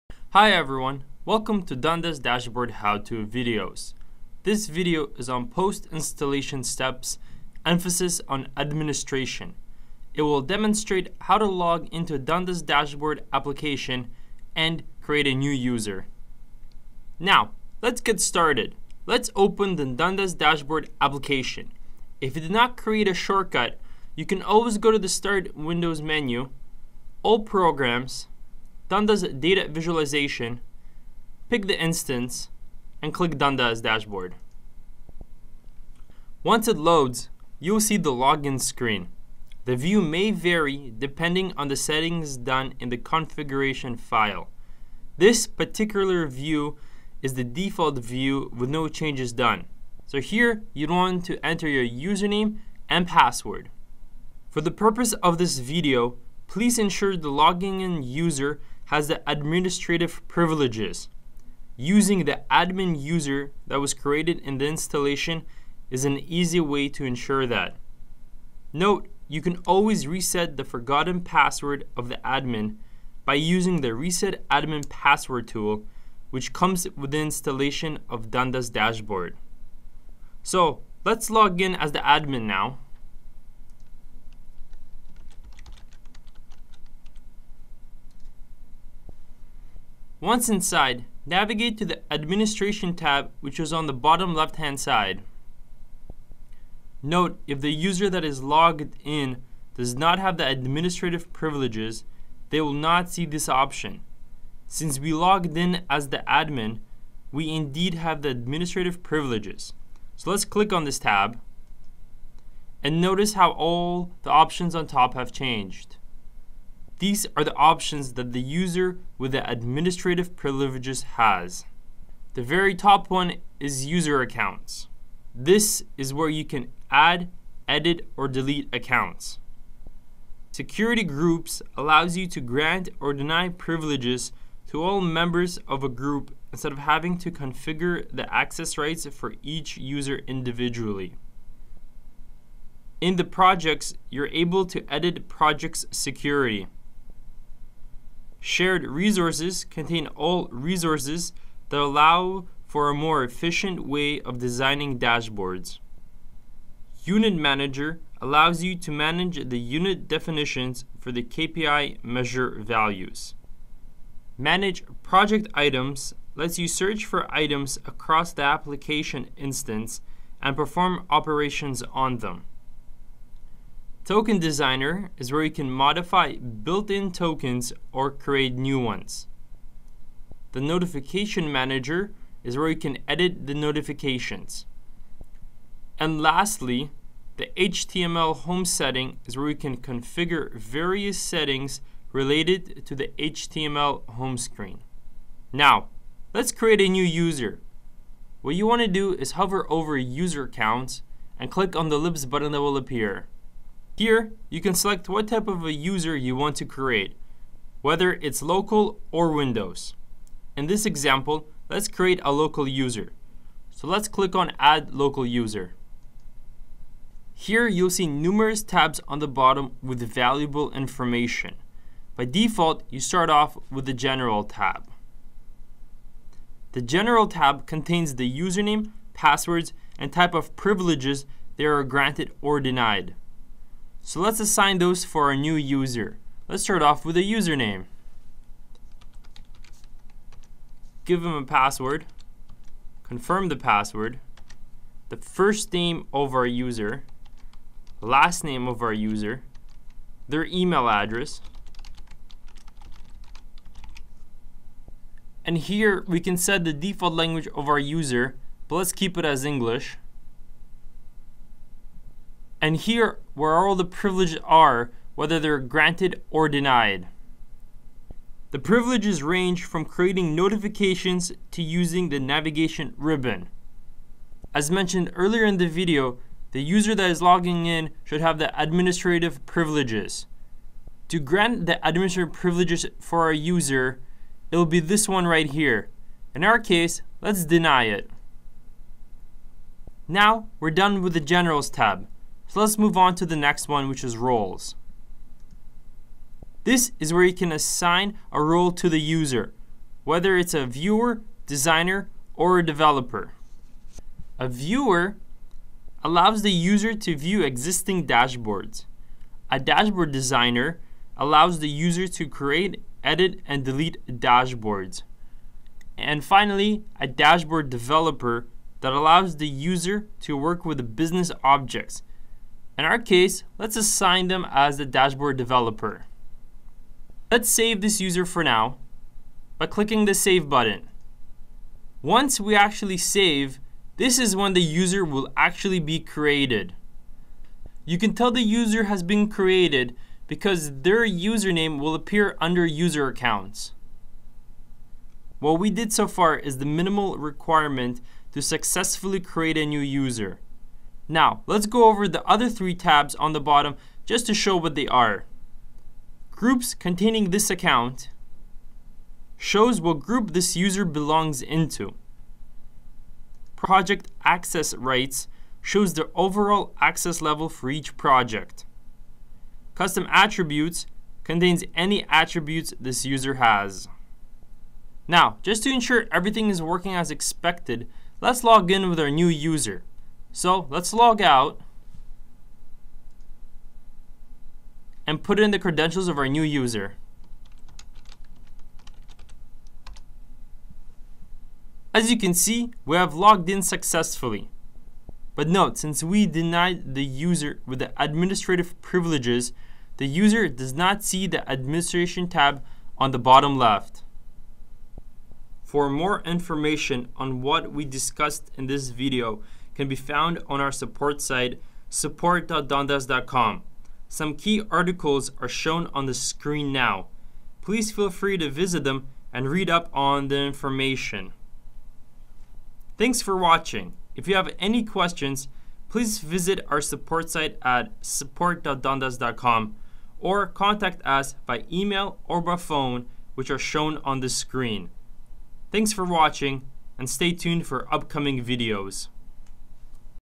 How To: Post Installation Administration (DD v. 4.0)
You've installed your copy of Dundas, but what do you do now?